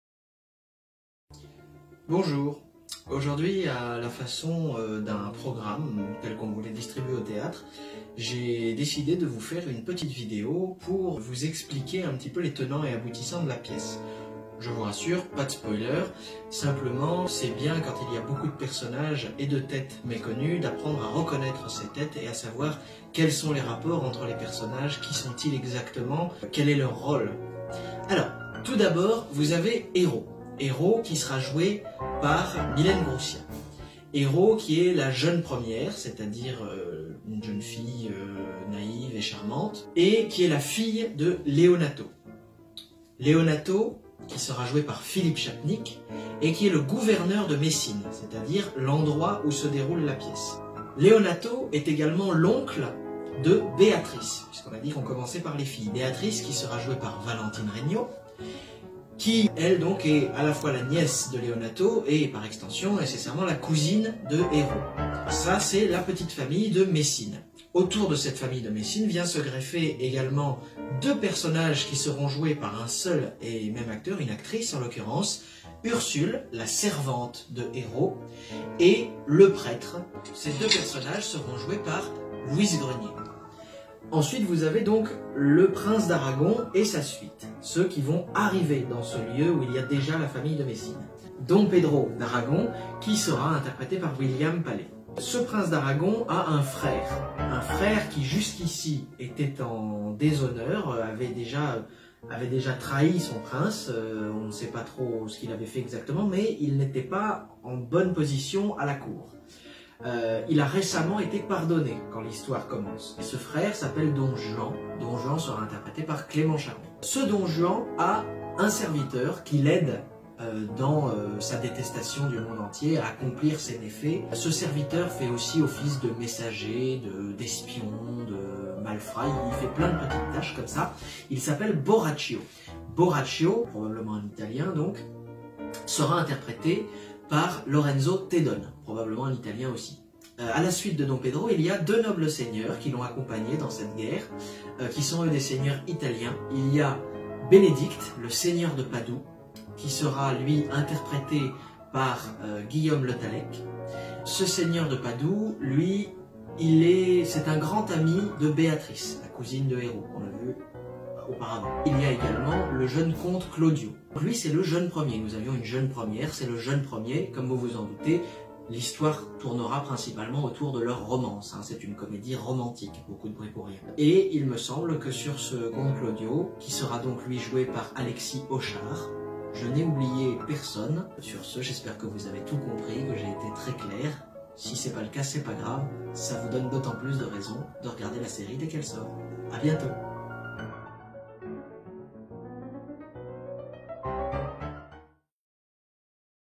''Play !'' - Présentation des Personnages
Simon Alteirac interprète du Narrateur, co-auteur et metteur en scène du Projet ''Play !'', vous présente les personnages de la pièce !